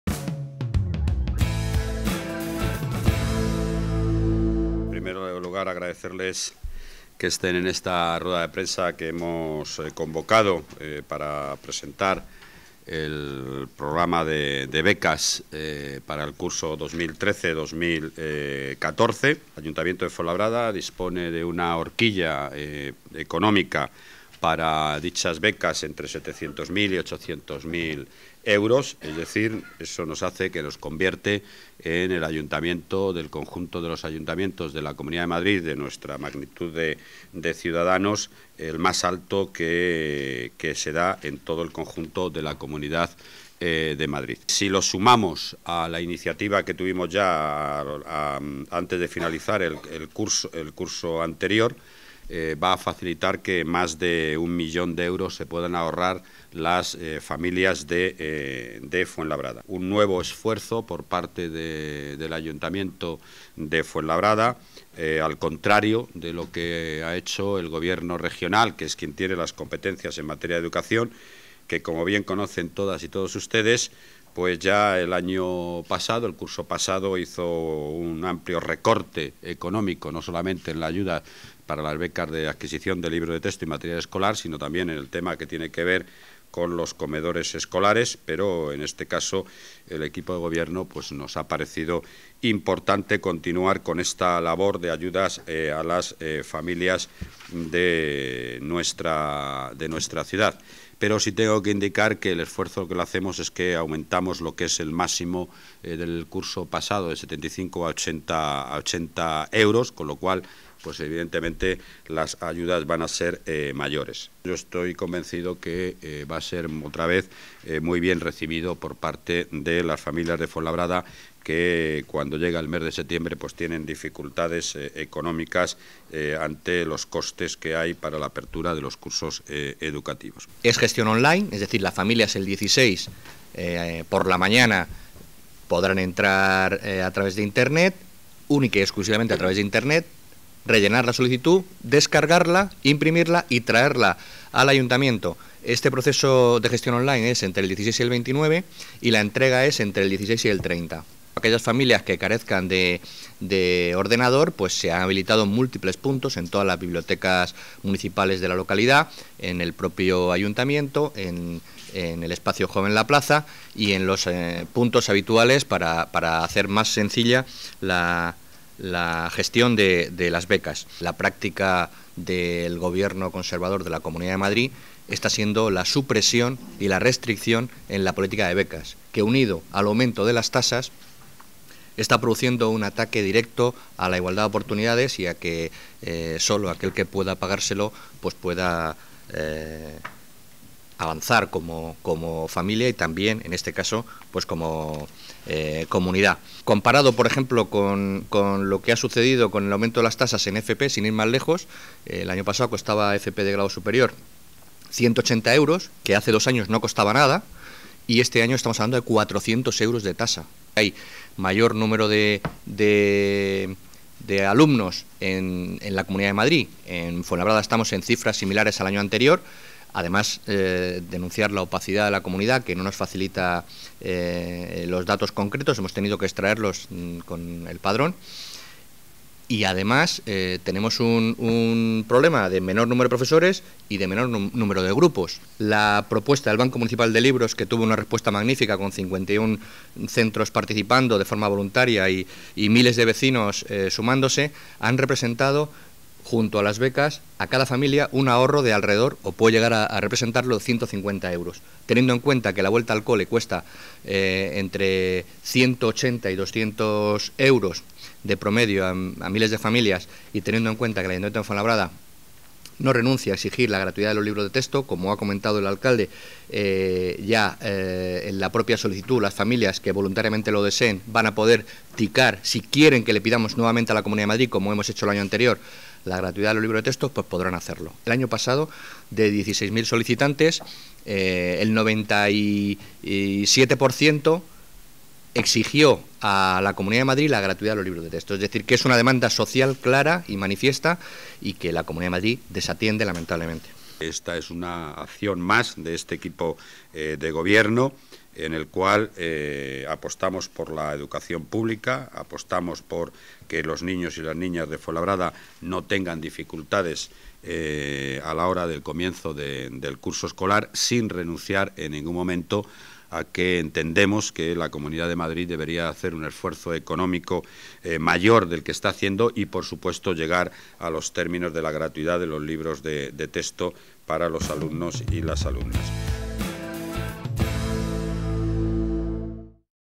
Presentación de los programas FUENBECAS y el Banco de Libros - Curso 2013-2014
El alcalde de Fuenlabrada, Manuel Robles, acompañado por el concejal de Educación, Isidoro Ortega, presentan y explican el programa de becas escolares para el curso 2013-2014 y el Banco de Libros.

Ambos representantes señalan que la cuantía aportada por el Ayuntamiento crece este año. Este hecho, sumado al proyecto del Banco de Libros puesto en marcha por el Ayuntamiento, conseguirá un ahorro sustancial para las familias fuenlabreñas.